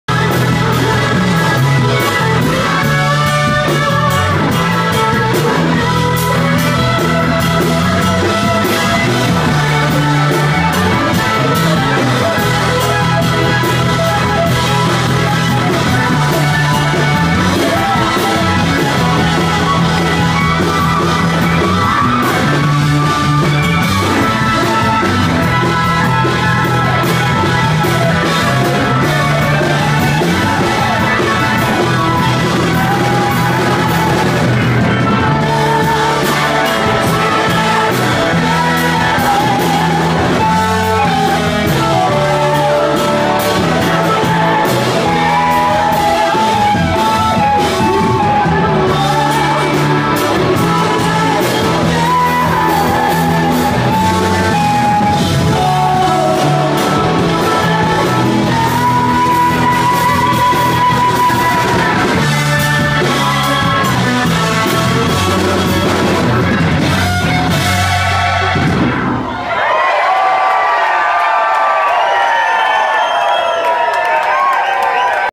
The Congress, Cervantes 8.28.15
Killin it for their last hometown Denver show